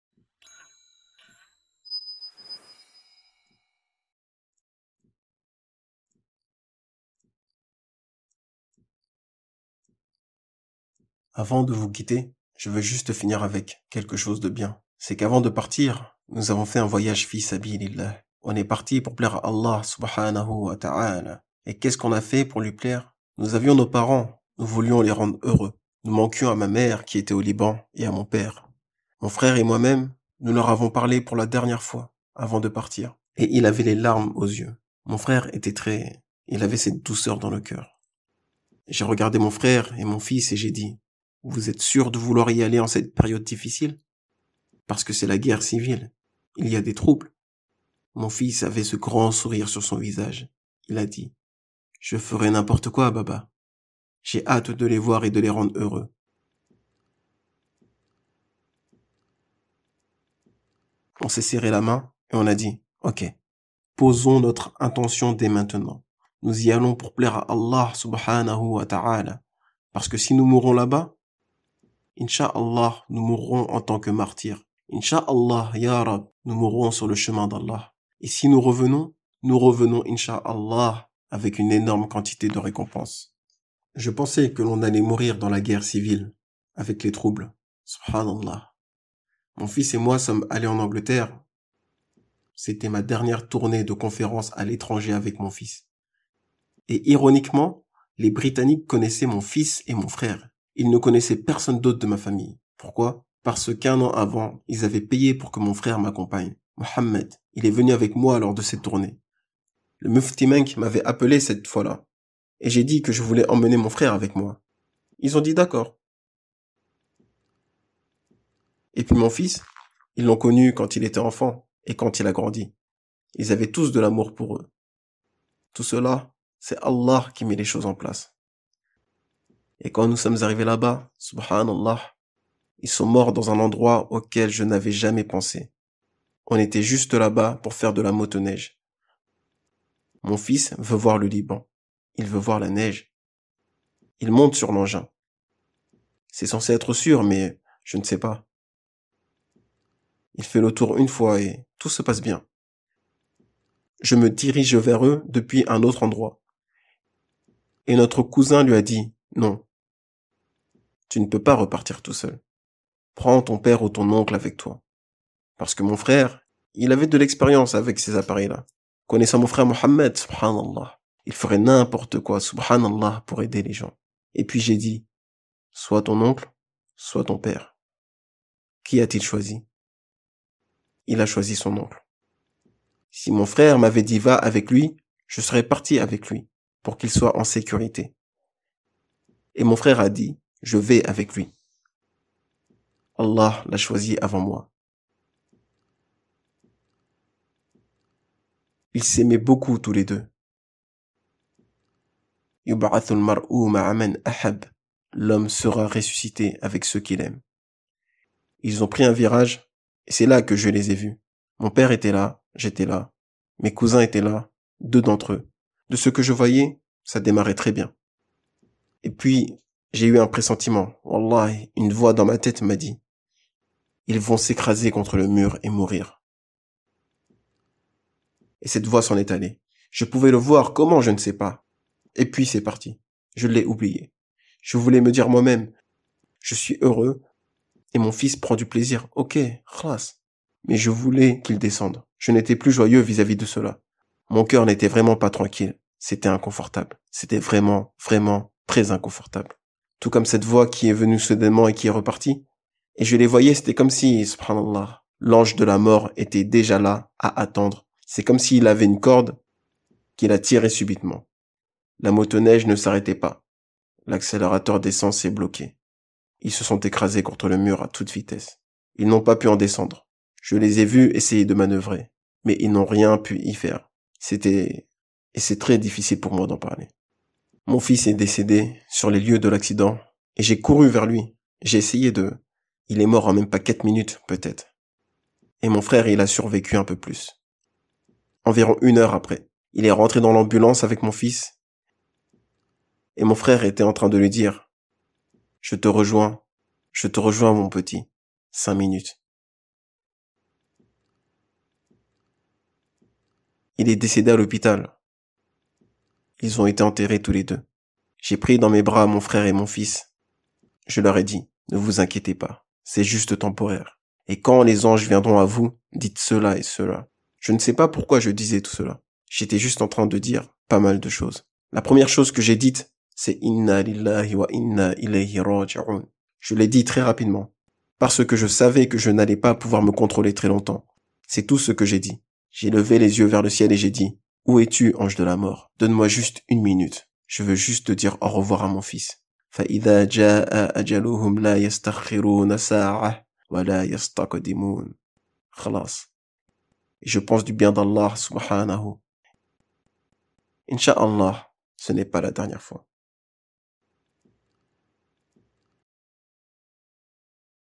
Ep 2: J'ai Perdu mon fils | Bilal Assad
Avant de vous quittez, je veux juste finir avec quelque chose de bien ! Et c'est qu'avant de partir nous avons fais un voyage Fii Sabiilillah...

💯 COMMENT NOUS SOUTENIR ?

Vidéos de FreeQuranEducation dans d'autres langues:
L'ÉDUCATION GRATUITE DANS D'AUTRES LANGUES :